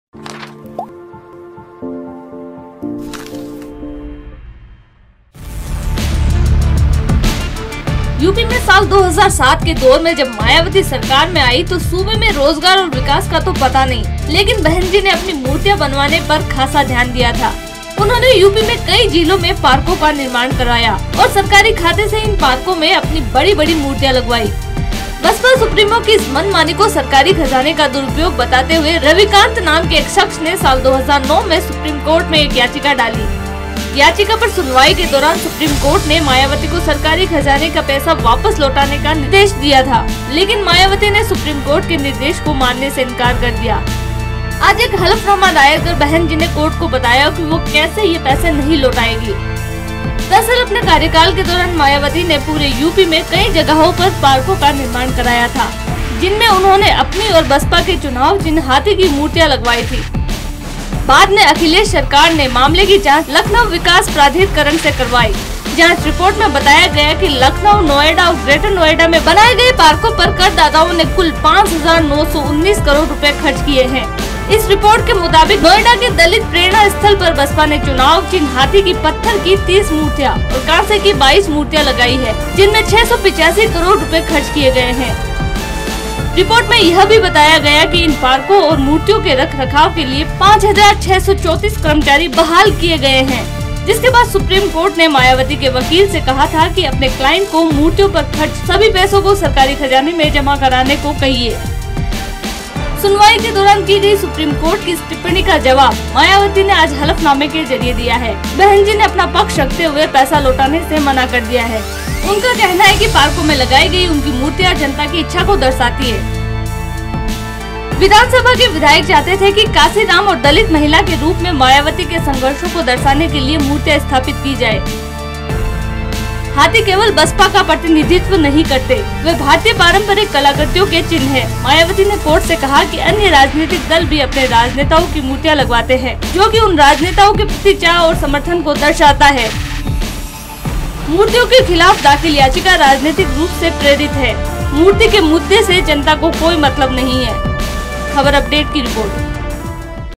Supreme Court के आदेश के जवाब में क्या कहा Mayavati ने ? | Khabar Update
2009 में मायावती के खिलाफ दायर याचिका की सुनवाई के दौरान सुप्रीम कोर्ट ने  कहा था कि मायावती मूर्तियों मे लगाए 5000 करोड़ रुपये वापस करे. देखिये इस पर मायवती ने क्या कहा है?